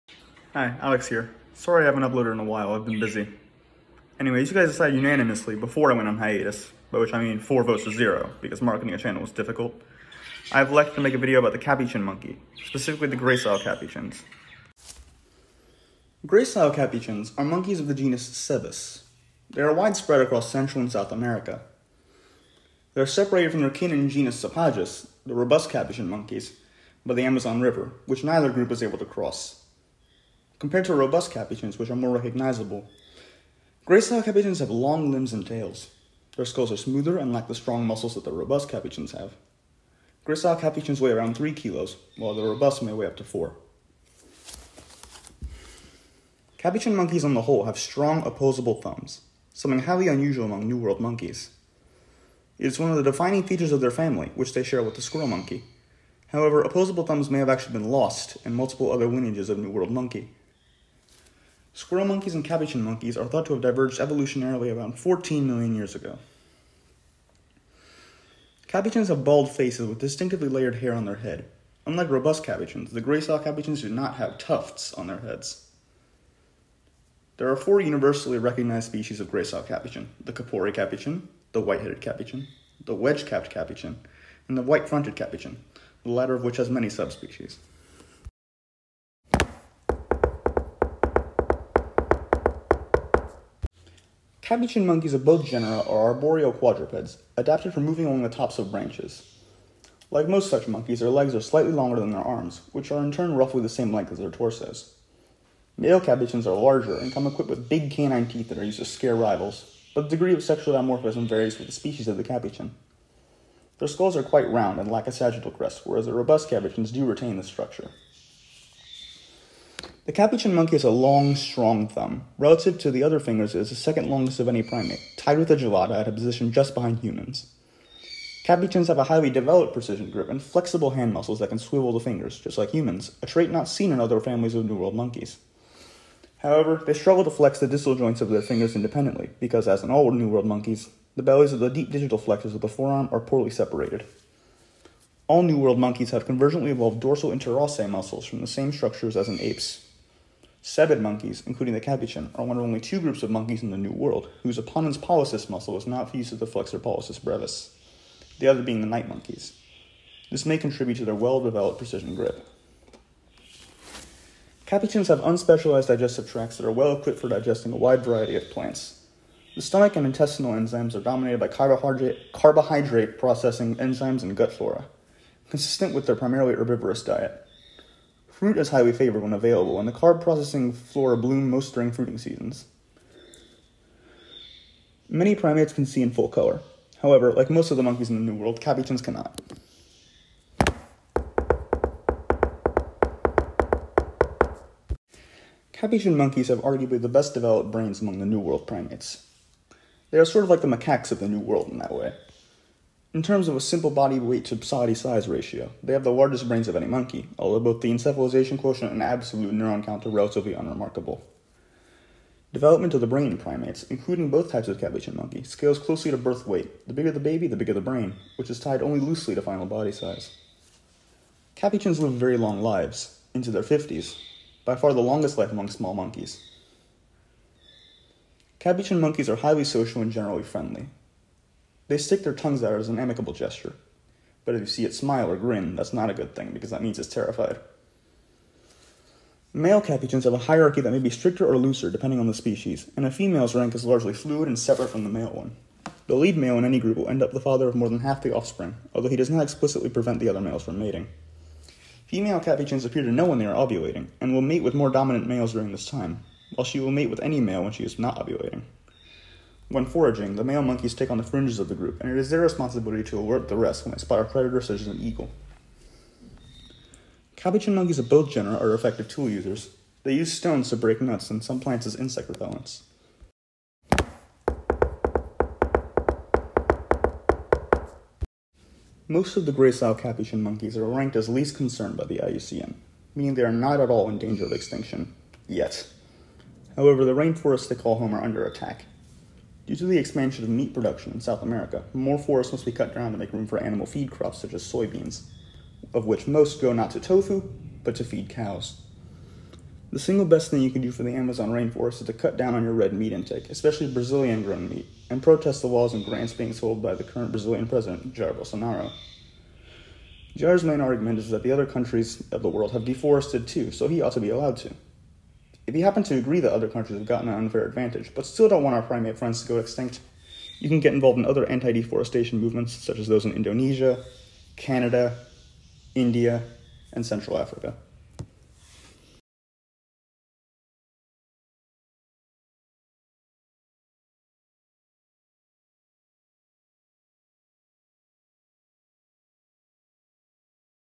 Upon the Capuchin Monkey (Cebus)
Glossary:
Dorsal Interossei- a set of hand muscles that lie along the back of the palm in between the metacarpal bones. They are responsible for spreading the fingers and are important for gripping awkwardly shaped objects.
Opponens Pollicis- the muscle that brings the thumb into opposition.
Flexor Pollicis Brevis- a small muscle in the fleshy mass at the base of the thumb, which helps flex the thumb.
Encephalization Quotient- a measure of an animal’s brain size relative to the expected quantity for an animal its size. Once used to measure probable intelligence, it is now known to be outdated.
Neuron- the small brain cells responsible for synapses, a cellular process associated with thinking.